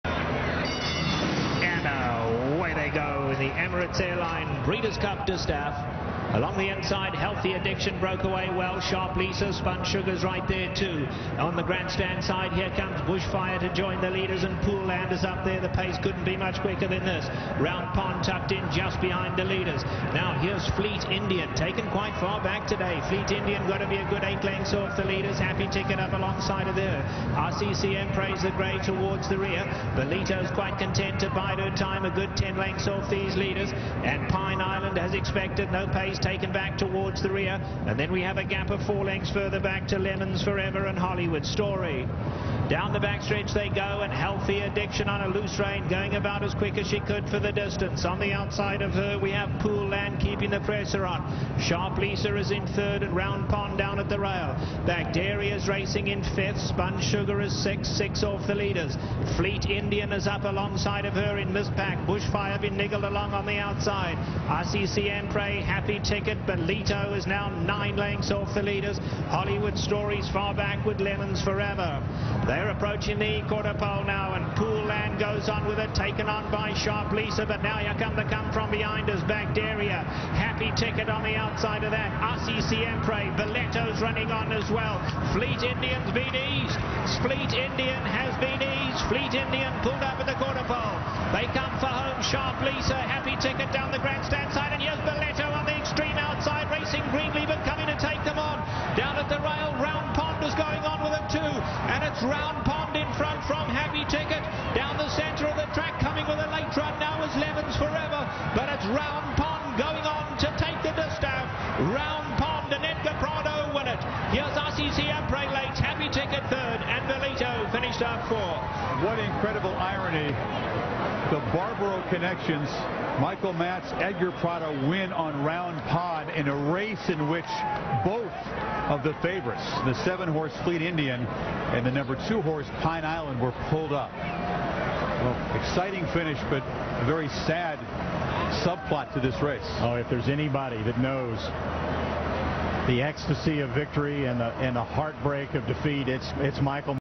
2006 Breeders' Cup Distaff - Round Pond
Fox Hill Farm's Round Pond wins the 2006 Breeders' Cup Distaff at Churchill Downs.